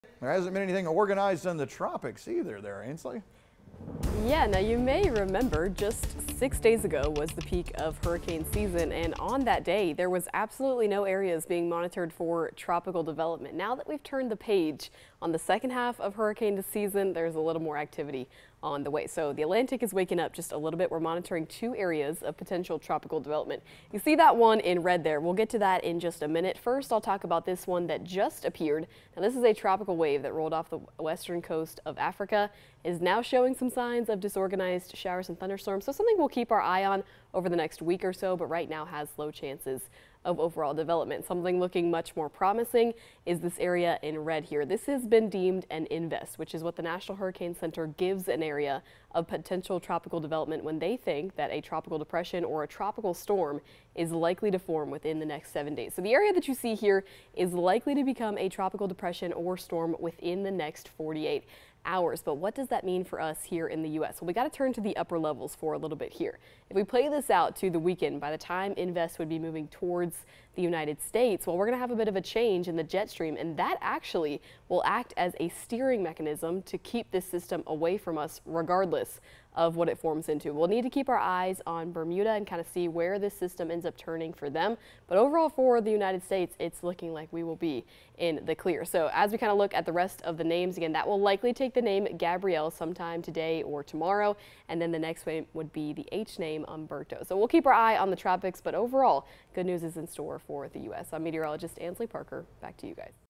Invest 92L likely to become Gabrielle, another area to watch hot on it's tail (5 PM Update 9/16/25)
The National Hurricane Center is monitoring two areas of potential tropical development in the Atlantic.